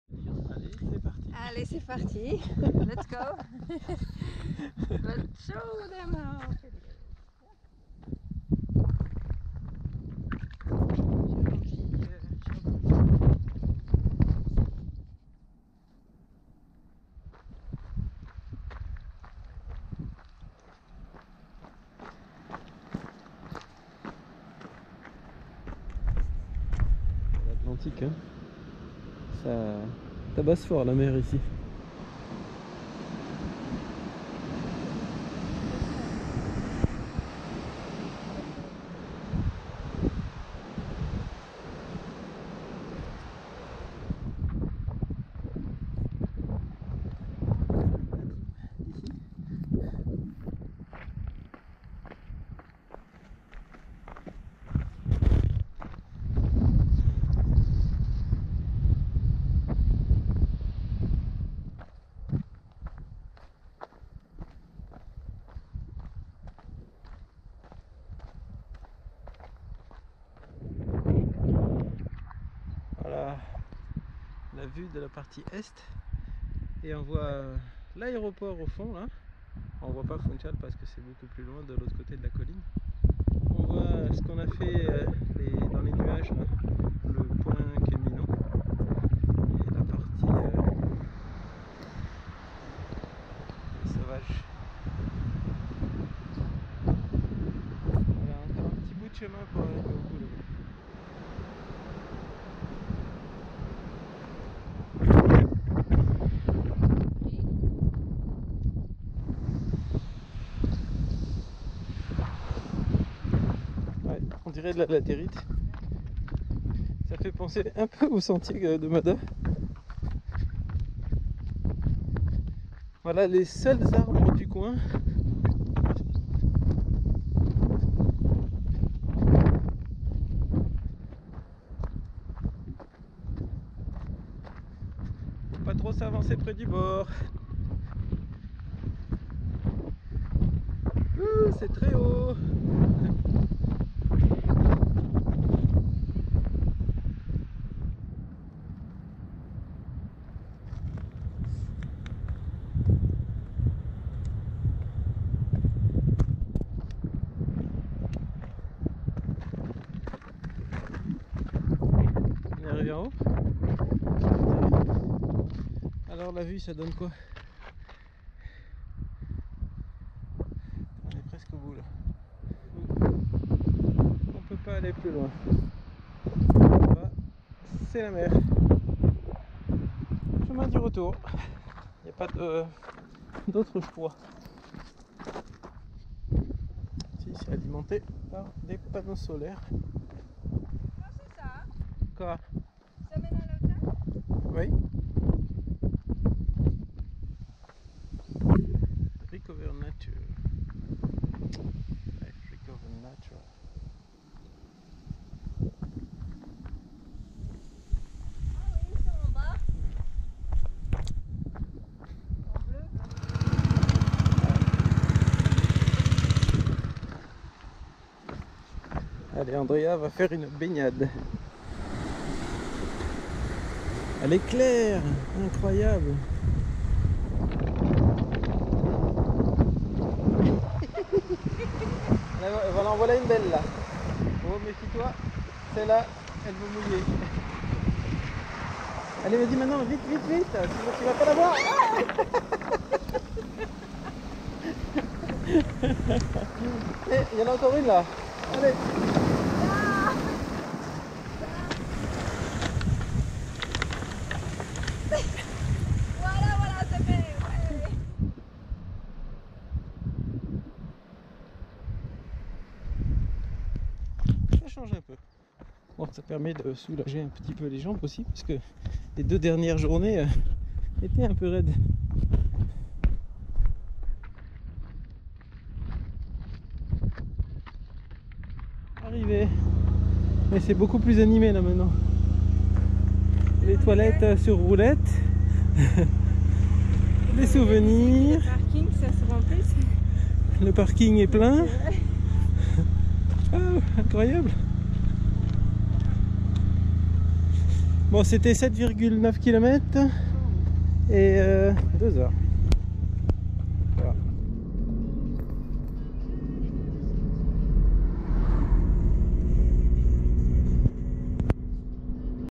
PONTO de Sao Lourenco
Aller Retour pointe extreme Est de Madeira. Trail de 8 km et 450 m D+. Paysage tres sauvage découpé par l'Atlantique